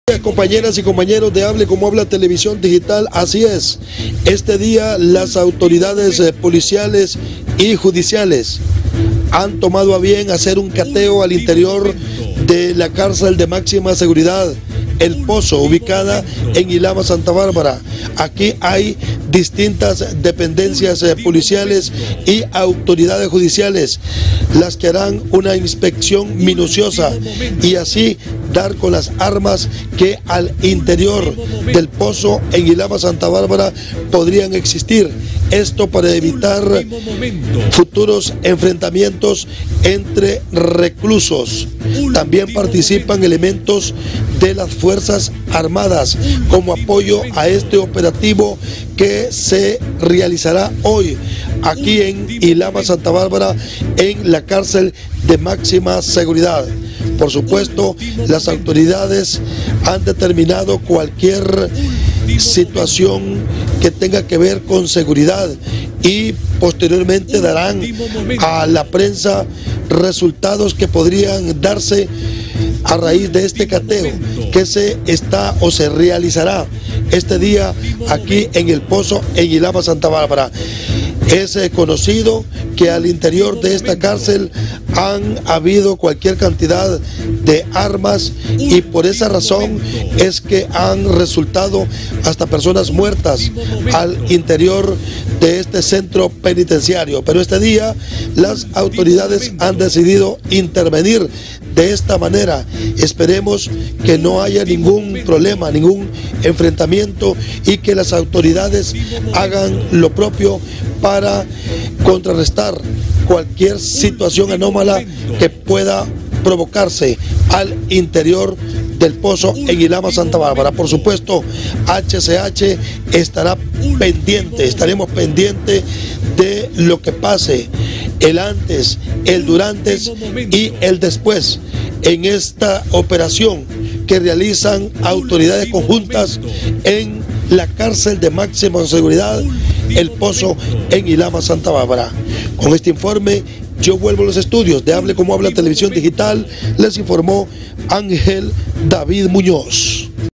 ¡Operación Refundación! Con 2,500 policías buscan recuperar gobernanza en cárcel "El Pozo"
Con el desplazamiento de al menos 2,500 efectivos policiales, elementos militares, helicópteros, patrullas y camiones, se desarrolla la "Operación Refundación" en el Centro Penitenciario de Ilama,  Santa Bárbara en el occidente de Honduras.